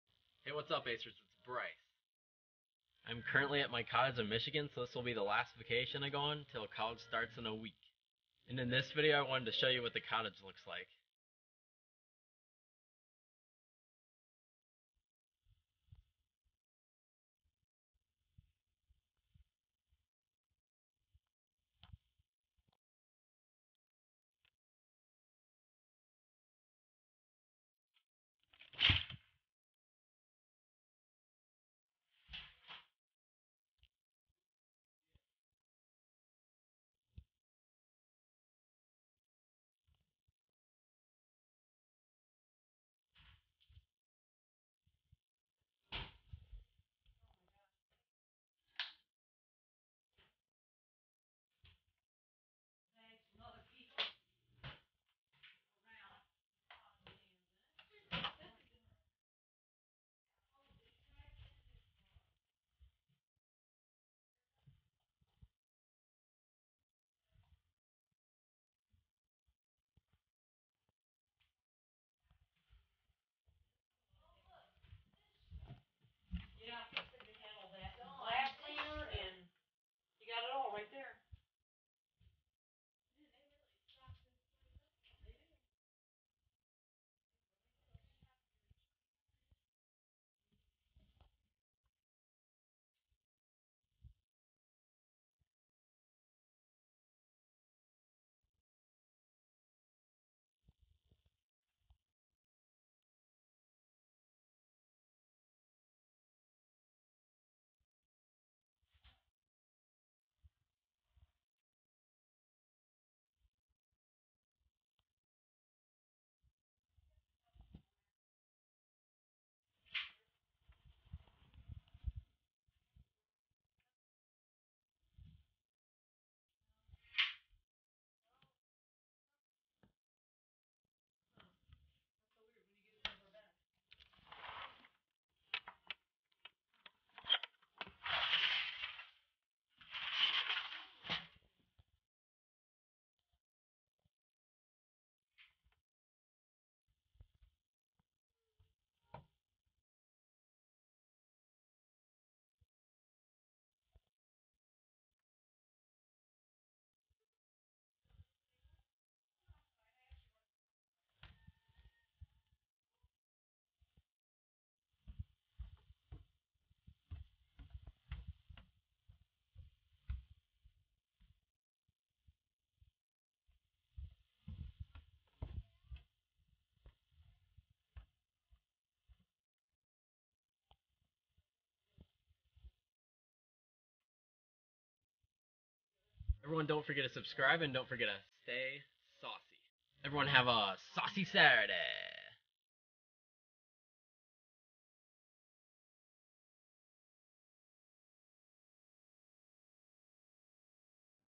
Michigan Vacation Cottage Tour
A tour of the cottage I stayed at in Shelby, Michigan for vacation.